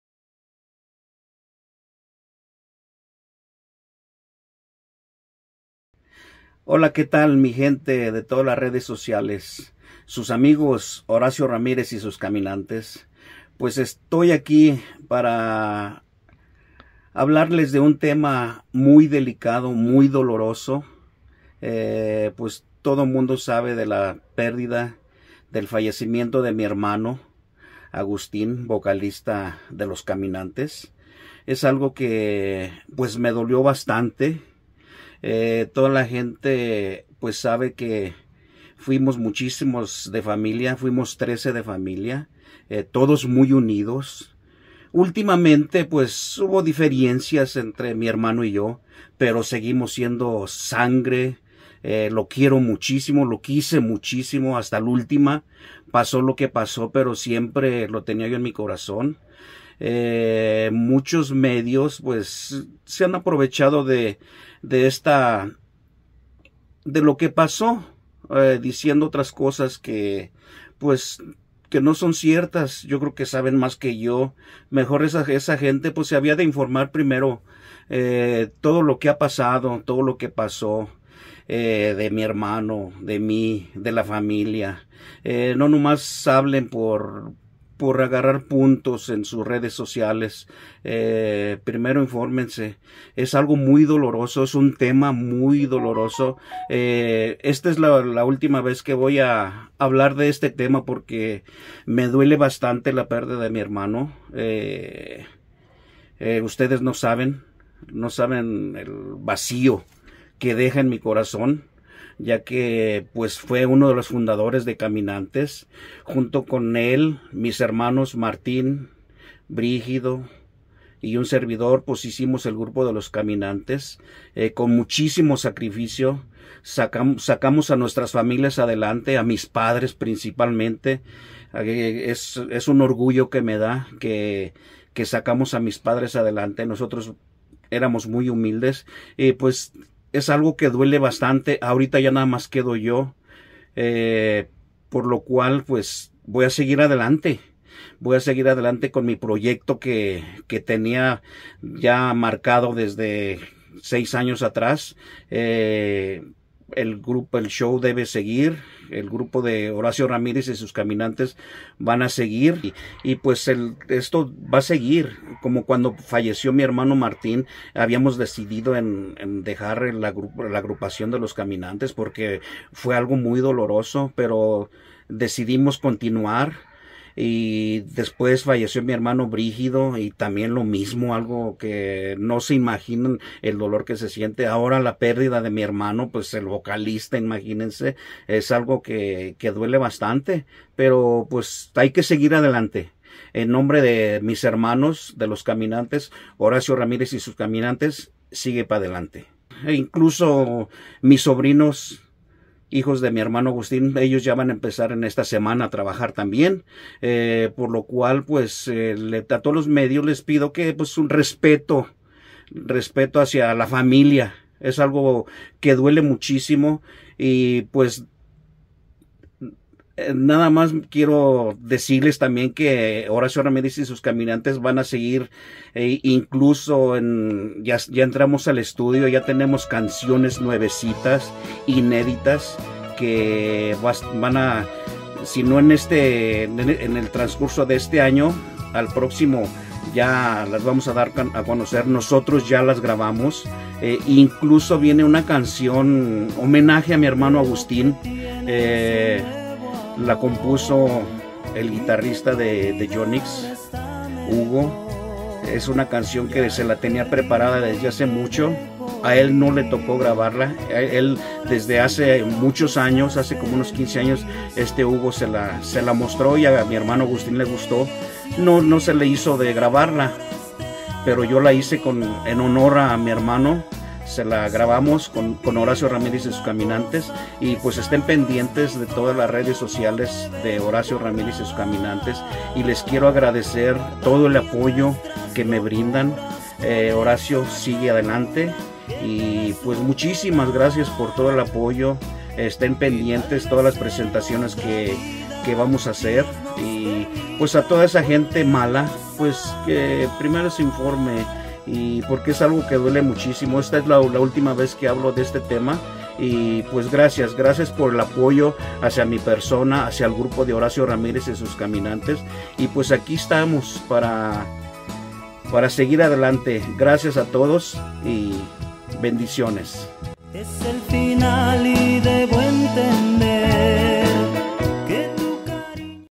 Ultima Aclaración de Horacio Ramírez.
Recientemente se ha dado a conocer "información" por supuestos medios de comunicación que solamente los engañan a ustedes. En este video exclusivo del canal de Horacio Ramirez Y Sus Caminantes les aclaro todas sus dudas. Muchos medios me han tratado de contactar para que hable sobre esta situación, pero con este video será la única vez que hablaré sobre ello.  Muchas gracias por su apoyo.